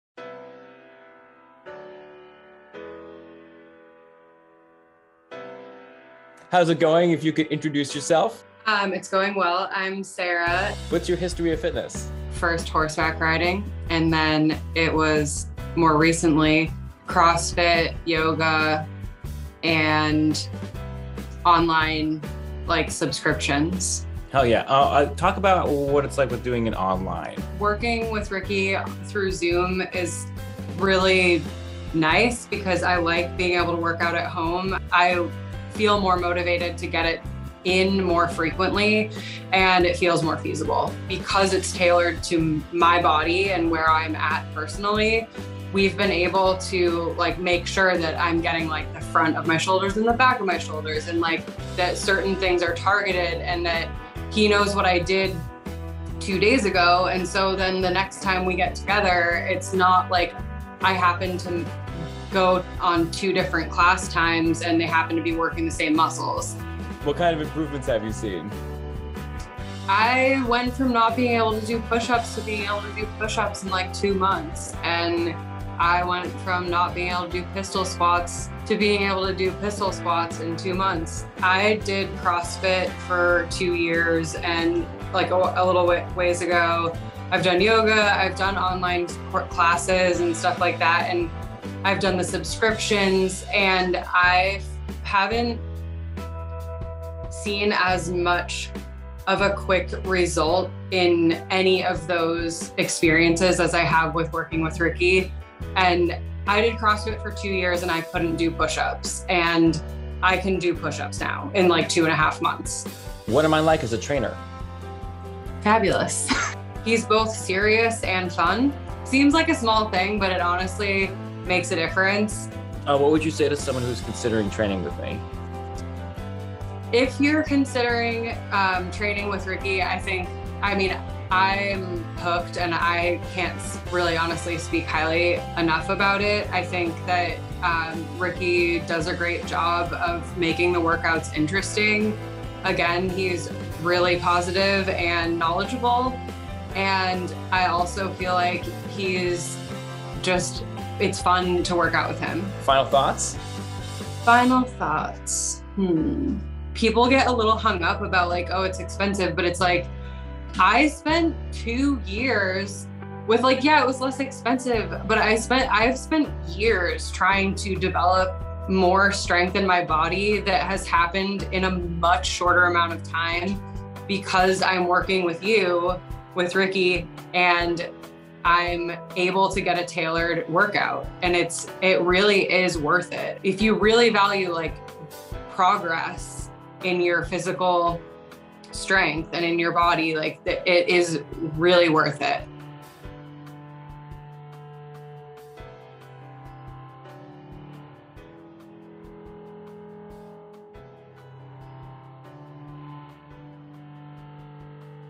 Saras Interview
Sara came to me frustrated with the lack of progress she had seen over years of fitness classes, ready for a change and some real progress. After two months, we accomplished her goals of pushups and pistol squats. her journey just goes to show what real focused training can accomplish.